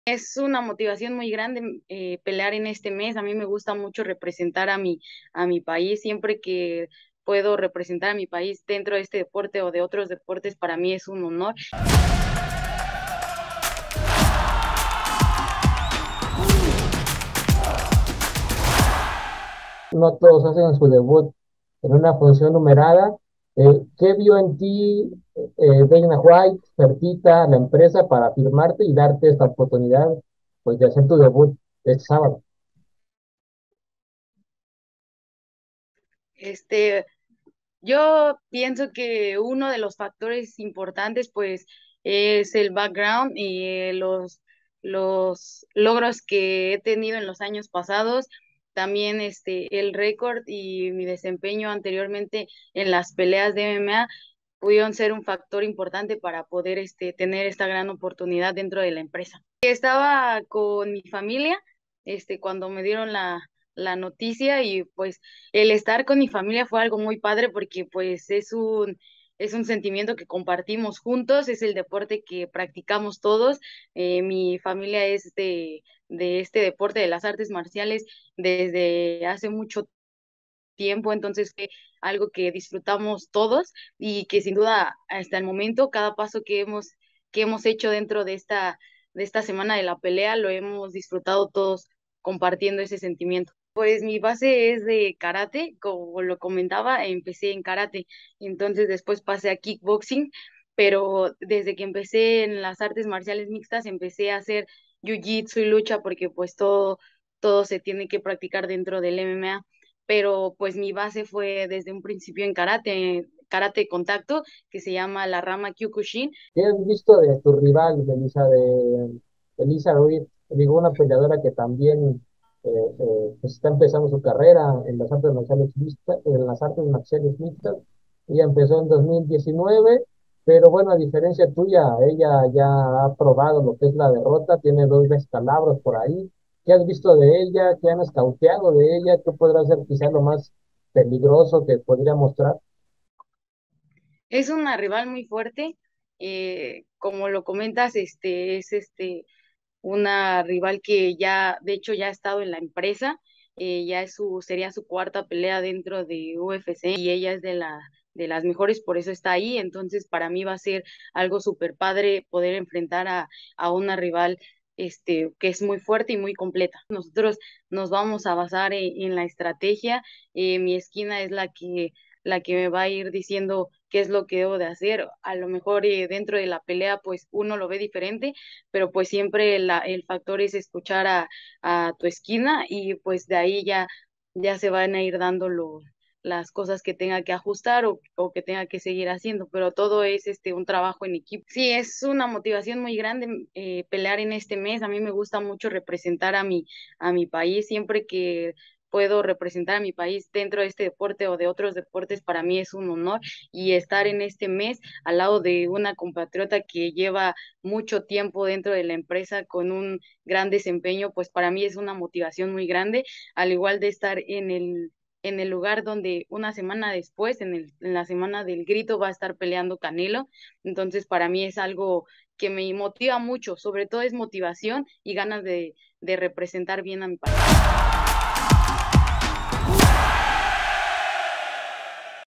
Así se prepara la luchadora de MMA previo a su debut en la UFC
Melissa ya se alista para debutar en el octágono de la UFC; donde prepara sus estilo de pelea y se analiza a su contrincante Elise Reed.